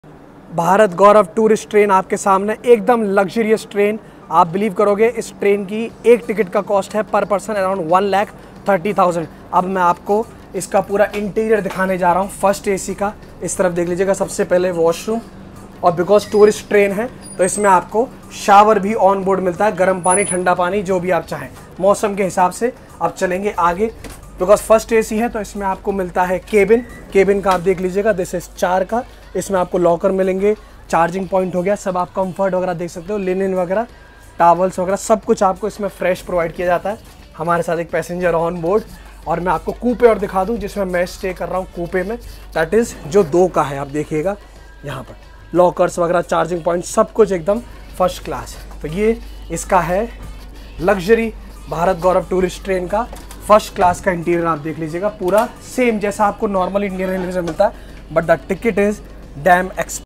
MOST LUXURIOUS TOURIST TRAIN JOURNEY IN INDIA | 3LACS ki ticket 😳😳indiarailways
MOST EXPENSIVE luxurious TRAIN journey TICKET. BHARAT GAURAV TOURIST TRAIN FIRST CLASS AC Luxurious FULL TOUR | Bharat Gaurav Deluxe AC Tourist Train under EK BHARAT SHRESTHA BHARAT Scheme “North East tour departed on 21s March from Delhi Safdarjung. Tourist Train consist of 4 first AC coaches, 2 second AC Coaches, one well equipped pantry car and two rail restaurants. It can accommodate total of 90 tourists onboard.